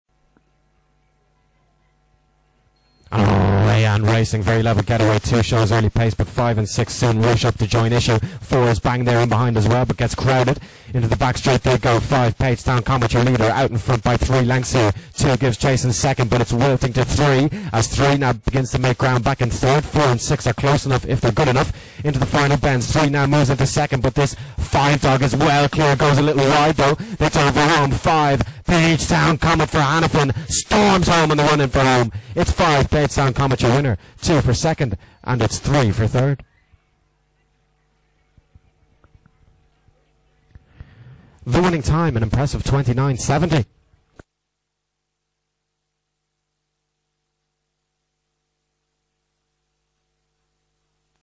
IGB - Happy Confirmation Brooke Weldon Reid A4 06/04/2019 Race 8 - Mullingar
Race: Happy Confirmation Brooke Weldon Reid   A4
Track Name: Mullingar
Date: 06/04/2019
Hour: 21:33

Results

1st    5     PAGESTOWN COMET     2/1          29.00
2nd    2     HOME MAI            2/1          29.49
3rd    3     AINTREE DASHER      3/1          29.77
4th    6     ARDILAUN MAX        6/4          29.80
5th    1     WINTER KATE         3/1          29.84
6th    4     MOTOR CHAPERAL      3/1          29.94

|MGR_2019_04_06_08